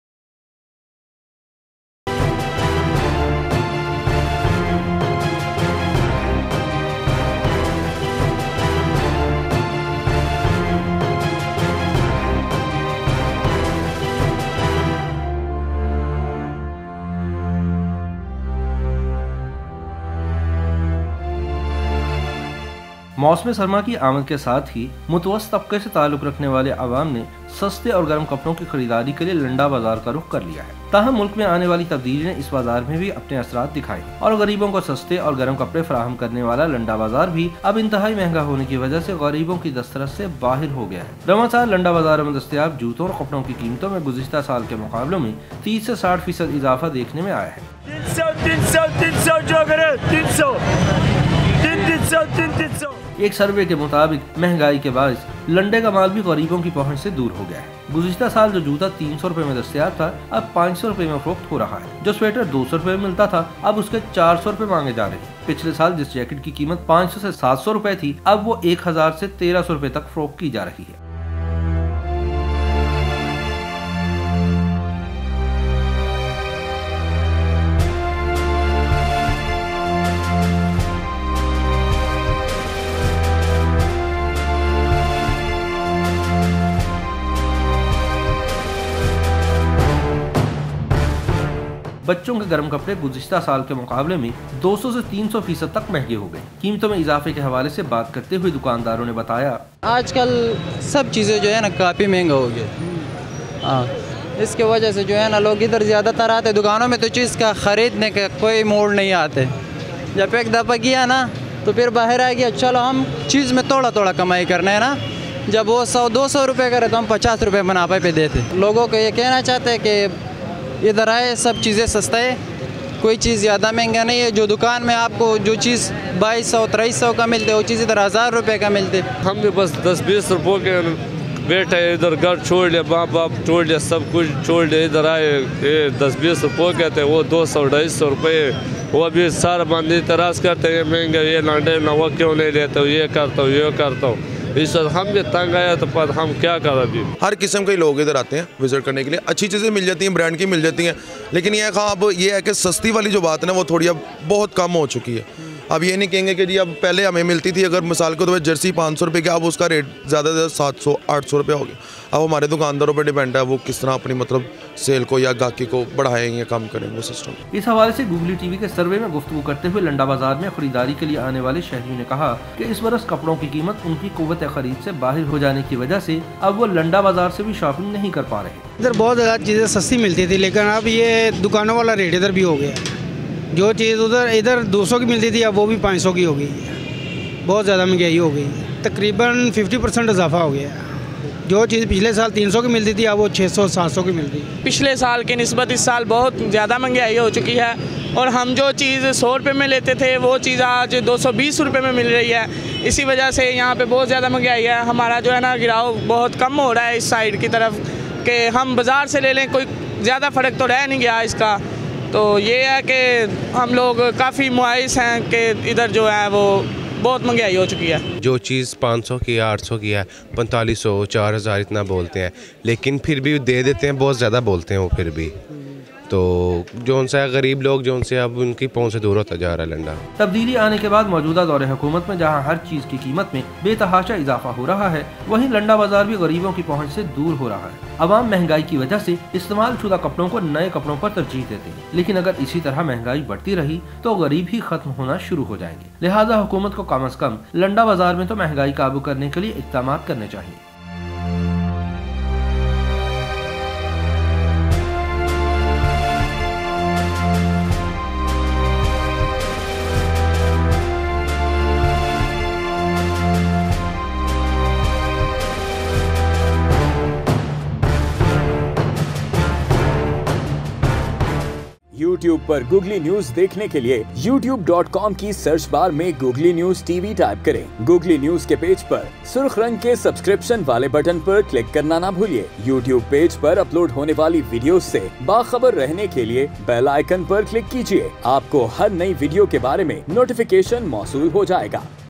Iss Baras Landa Bazar Bhi Gareebon Ki Quwat Khareed Sai Bahir Ho Gya | Googly News TV
Iss Baras Landa Bazar Bhi Gareebon Ki Quwat Khareed Sai Bahir Ho Gya